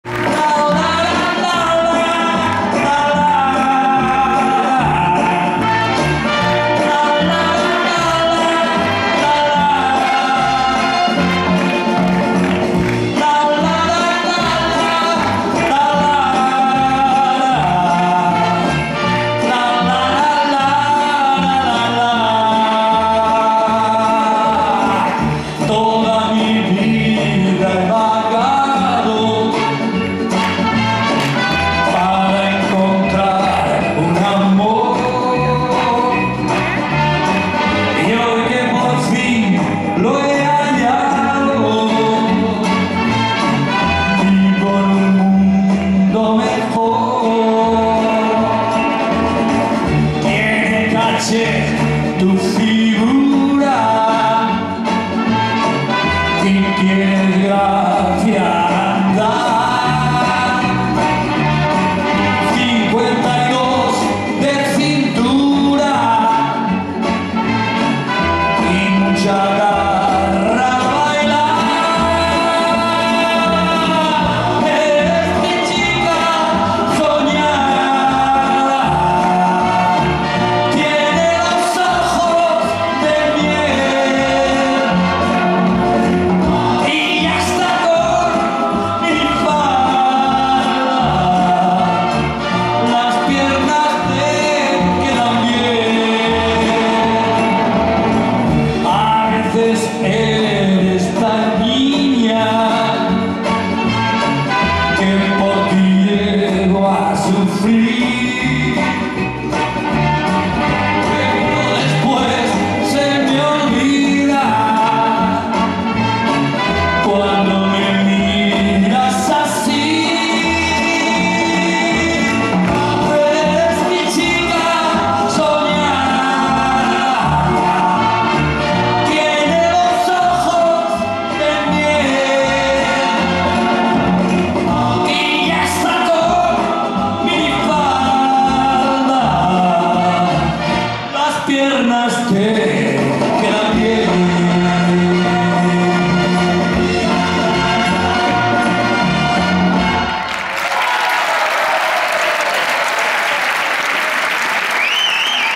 Emilio Solo - ?
Concierto caritativo Caritas en la Rambleta de Valencia el 23 de abril de 2024